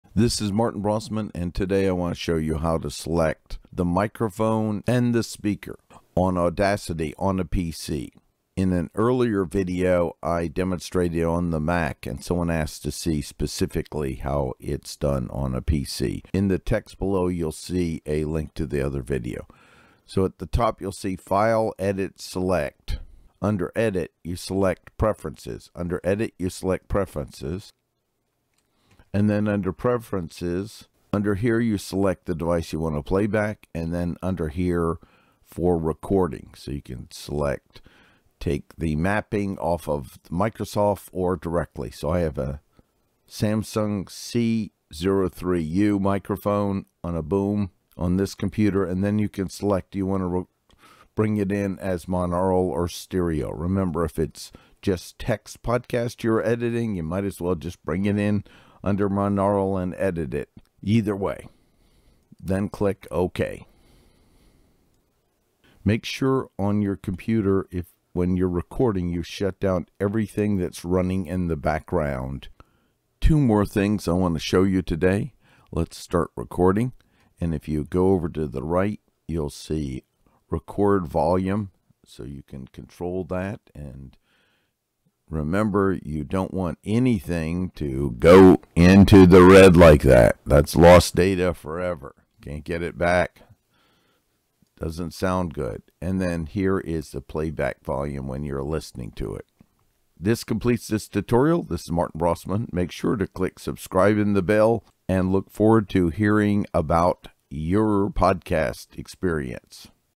Selecting the Microphone and Output speaker as well as Mic volume on Audacity for Windows
Another video about downloading as an MP3 and WAV!